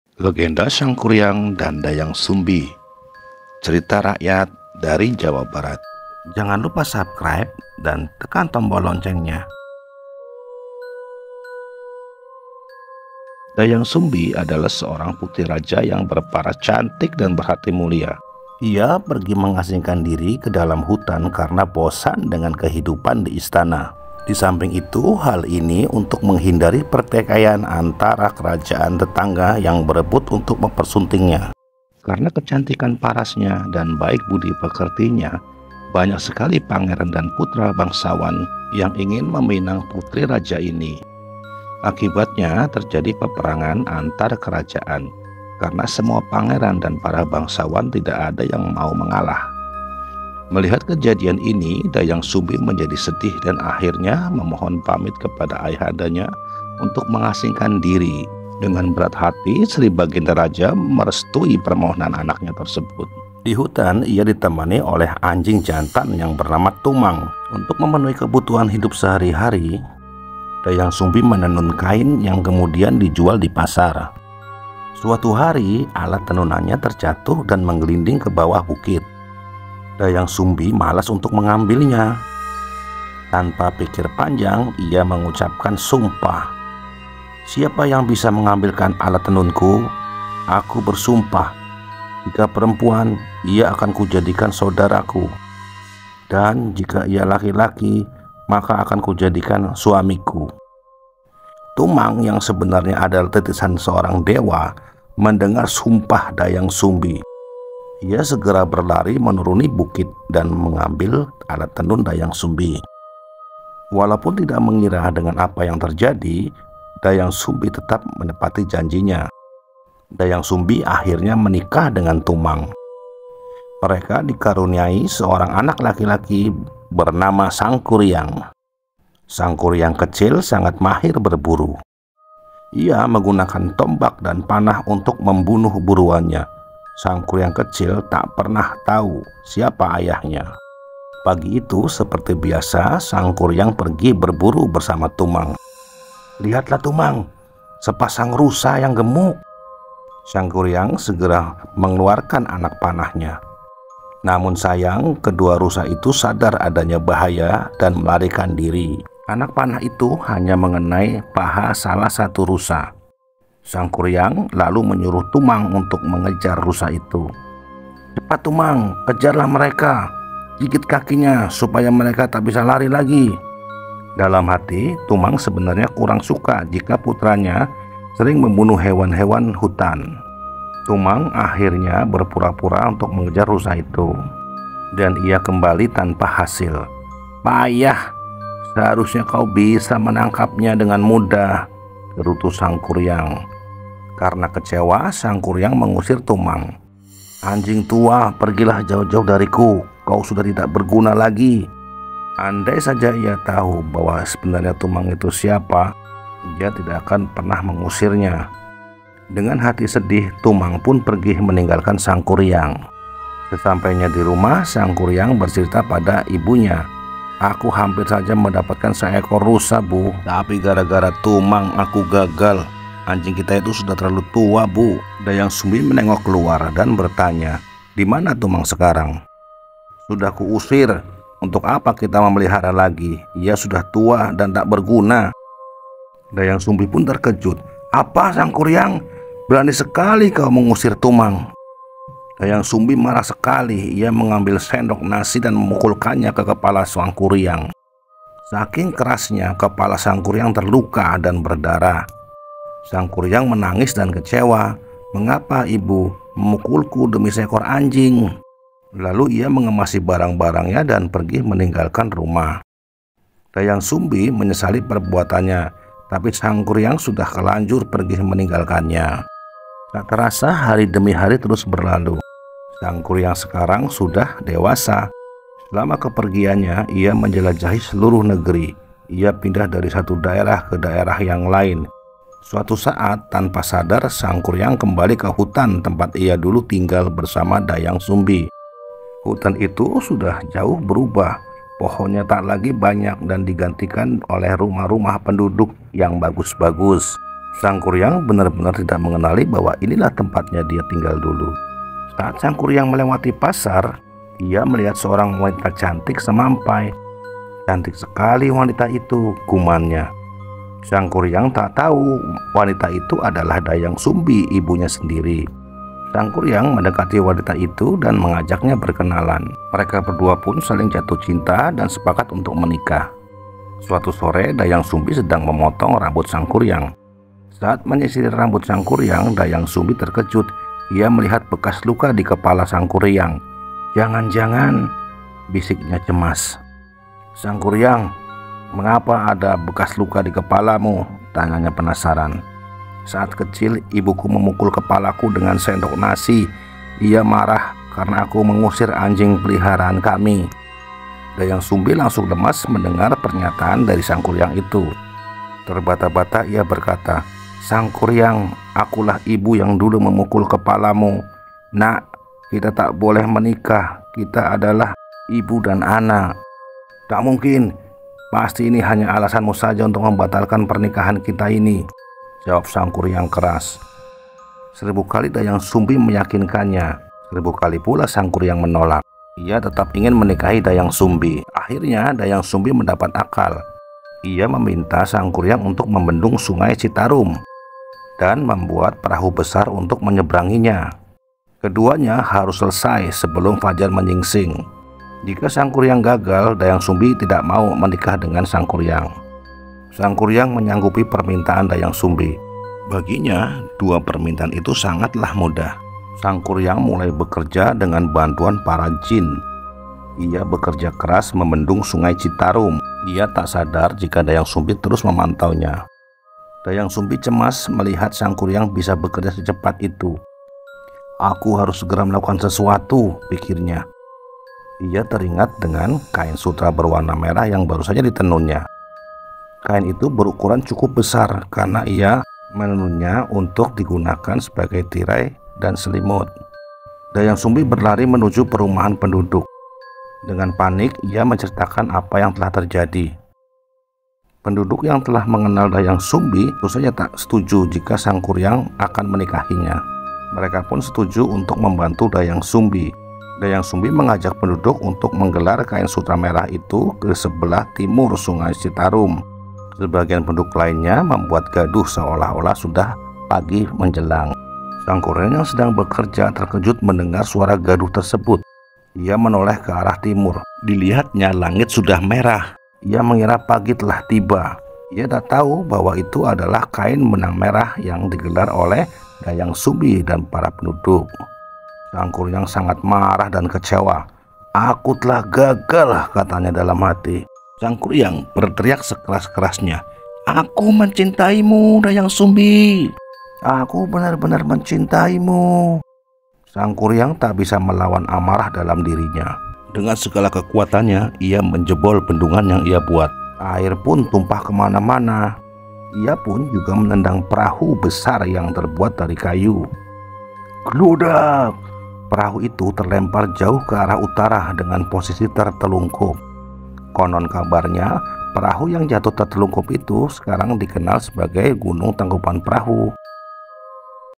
LEGENDA SANGKURIANG & DAYANG SUMBI ~ Cerita Rakyat dari Jawa Barat || Cerita Anak Nusantara
Pada zaman dahulu di sebuah kerajaan hiduplah seorang putri cantik yang bernama dayang Sumbi. Tak hanya cantik wajahnya, sikapnya juga ramah, sopan dan santun. Sehingga banyak putra kerajaan dan bangsawan yang ingin meminangnya. Akibatnya banyak terjadi peperangan antar kerajaan. Melihat itu akhirnya ia memilih keluar dari kehidupan istana dan hidup menyendiri di hutan. Ia hanya di temani seekor anjing jantan.

Untuk memenuhi kehidupan sehari-hari nya dia menenun. kisah kehidupan baru bermula saat dia tengah menenun lalu alat pintal nya terjatuh ke lembah. Tanpa sadar ia bersumpah, siapa yang dapat mengambil kan alat pintal ku jika dia wanita akan diangkat menjadi saudara dan jika dia laki laki maka akan di jadikan suami. Si anjing mengetahui sumpah Dewi dayang Sumbi segera berlari dan mengambil alat tenunan nya. Alhasil menikah lah dayang Sumbi dan anjingnya tersebut.

Pernikahan mereka di karunia seorang anak laki laki dan di beri nama Sangkuriang. Tak terasa Sangkuriang tumbuh remaja. Hobi nya adalah berburu binatang di hutan. Suatu pagi seperti biasa Sangkuriang pergi berburu di temani anjing nya. Saat itu ada 2 ekor rusa dan langsung di bidiknya. Bidikan nya meleset akhirnya kedua rusa itu pun lari. Sangkuriang memerintahkan anjingnya untuk mengejar.

Anjing nya yang tak lain adalah merupakan Ayah nya itu tak suka jika Sangkuriang sering berburu binatang di hutan akhirnya dia hanya pura-pura saja mengejar rusa itu. Alhasil pulang dengan tangan hampa. Melihat anjing nya gagal menangkap rusa, Sangkuriang marah dan mengusir anjing tersebut.

" Dasar anjing payah. Anjing tua. Pergilah dari ku. Kau sudah tak berguna lagi. Akhirnya anjing itu pun pergi meninggalkan Sangkuriang.

Ketika pulang kembali ke rumah Sangkuriang menceritakan hal tersebut kepada ibunya. Dayang Sumbi marah bukan kepalang. Dipukulnya kepala Sangkuriang dengan sendok nasi hingga berdarah. Lalu apa yang terjadi?

 Yuk ikuti Cerita nya di channel @ZeegersTv-be2uy

 Selamat menyaksikan